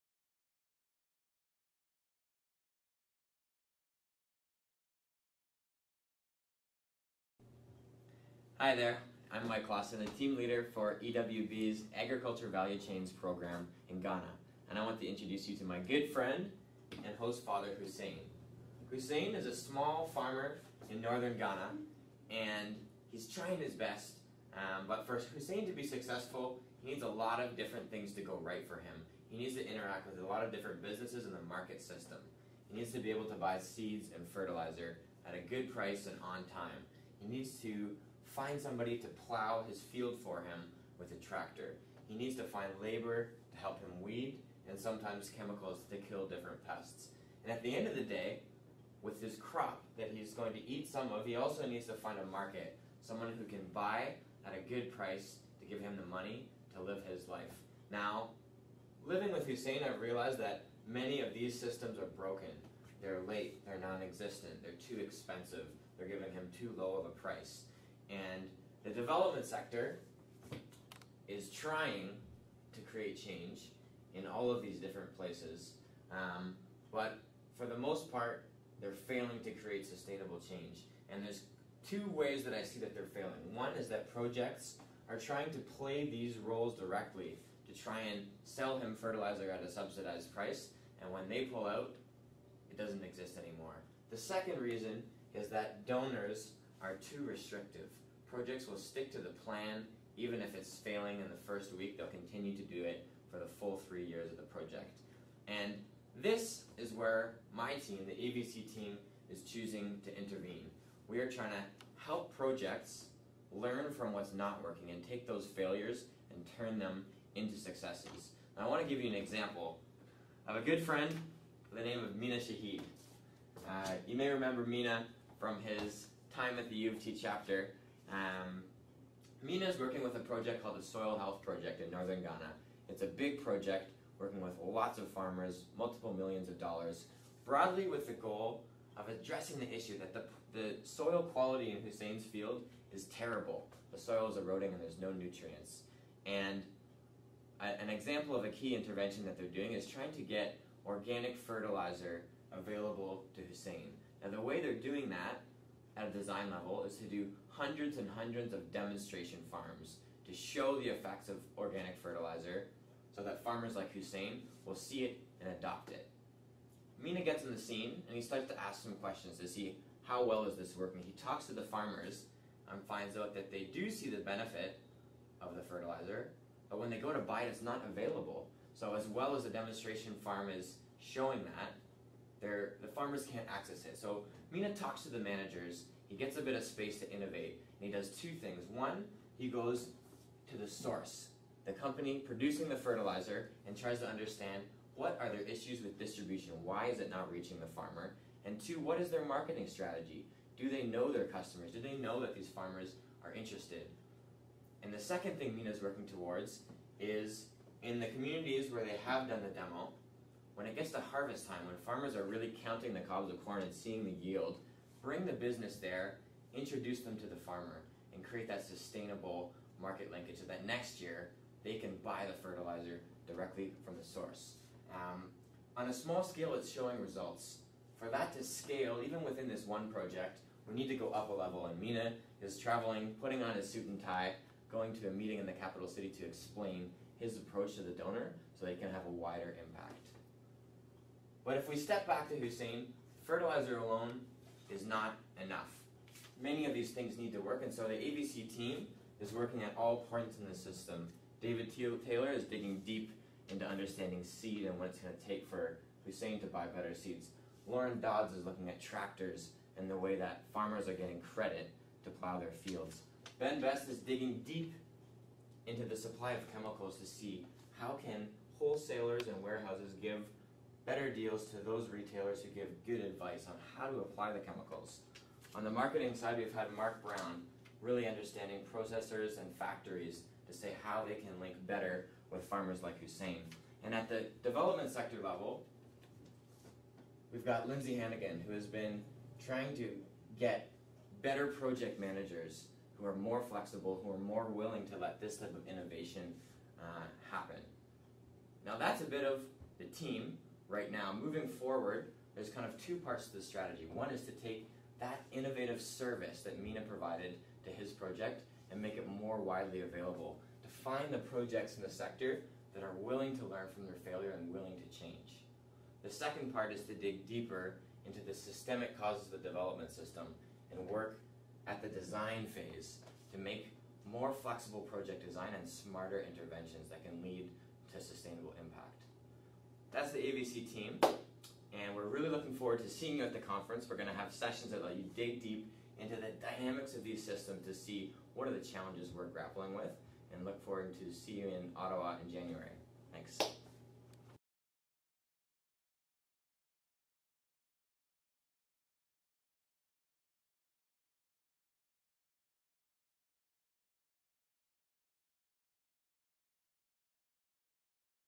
An Introduction to EWB's Ghana AVC Team with Mike Klassen.m4v
Mike Klassen introduces you to EWB's Agriculture Values Chain in Ghana!

Watch the video to learn about what the AVC team members do, their approach and some strategies for moving forward. They are excited to engage with you on their work at Conference in January.